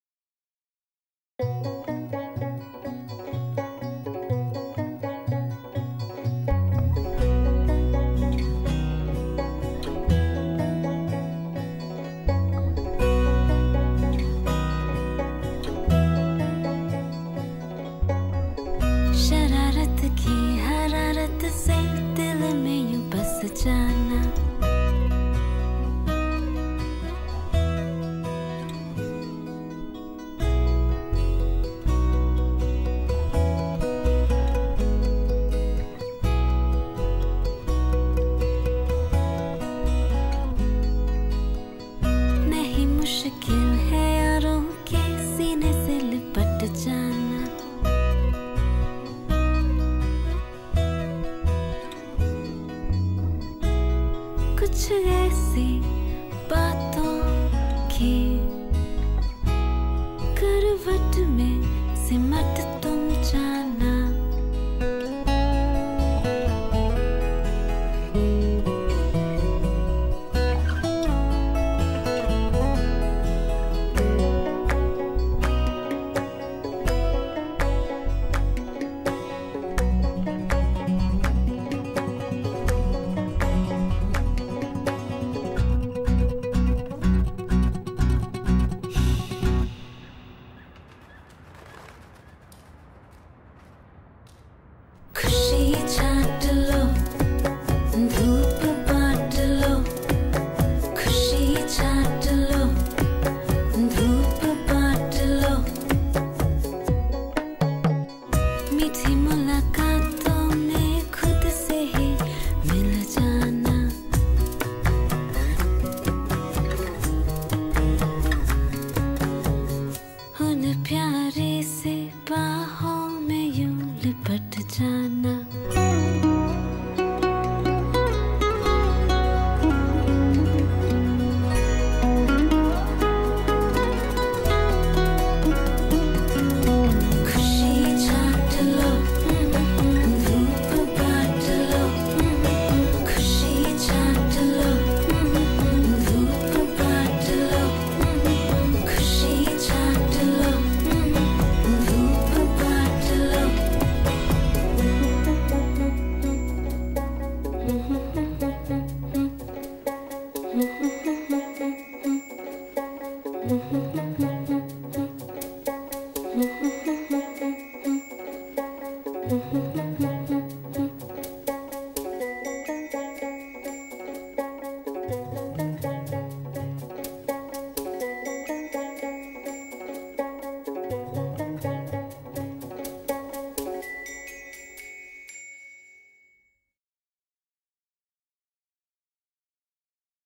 EZEE HUGS --- ADVERTISE BASED SHORT FILM
Ezee Hugs is a small step to provide warmth to underprivileged children during harsh winters. We will wash them with Ezee and deliver soft and fluffy sweaters to underprivileged children.